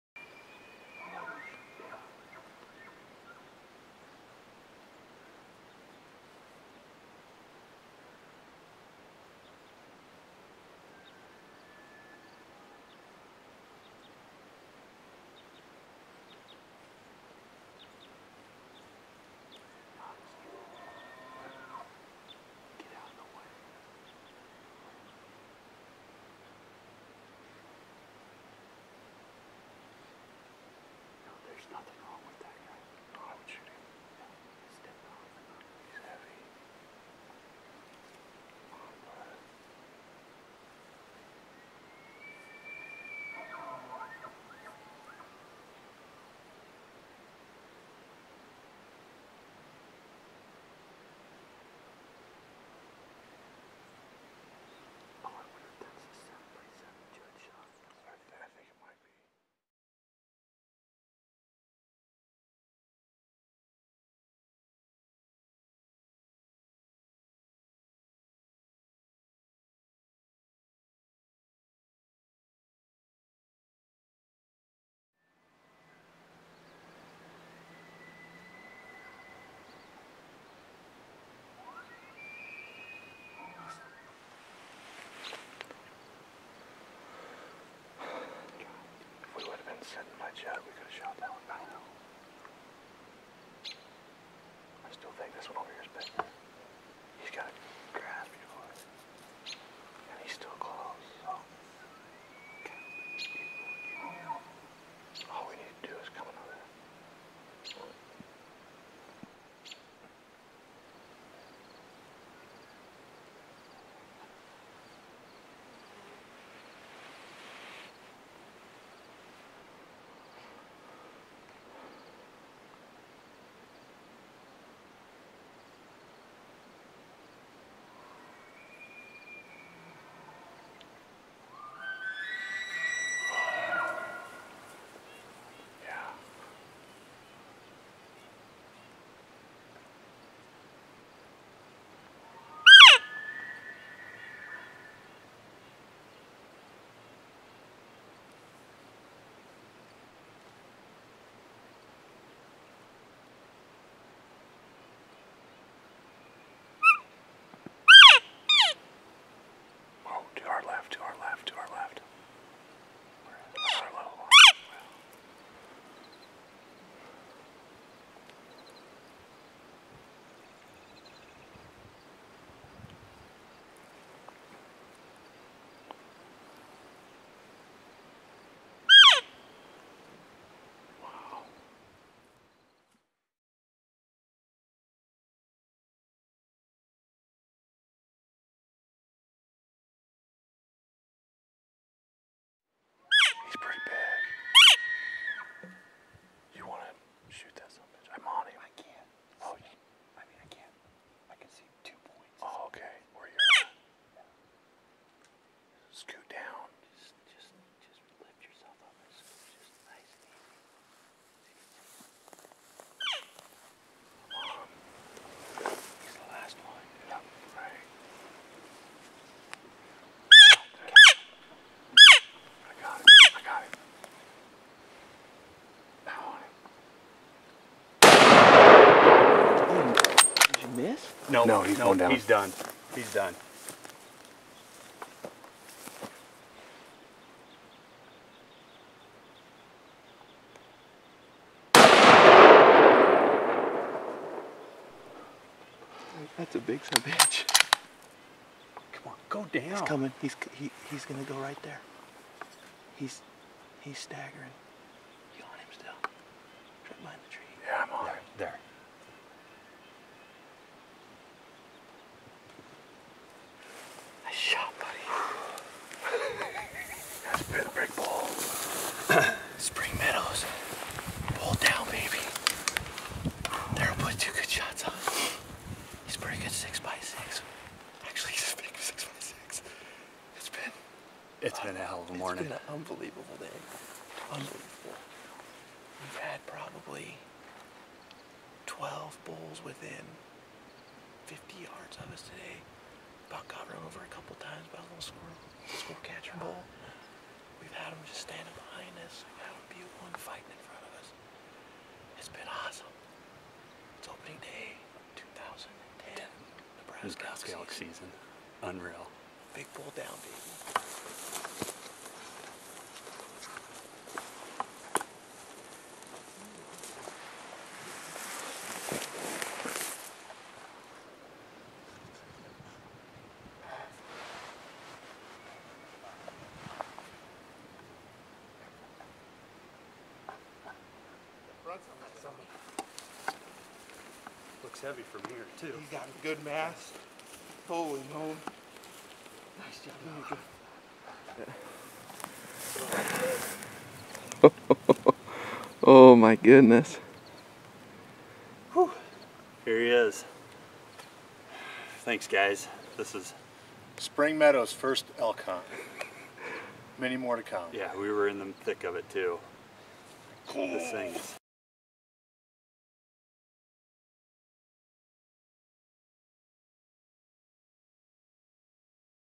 SpringMeadows Elk Hunt 2010
SpringMeadows Hunting & Fishing guided Elk Hunt on September 25, 2010. Guides Justin Simmons and Judd Lee. Hunter Daryl Dubas. Camera man Justin Simmons.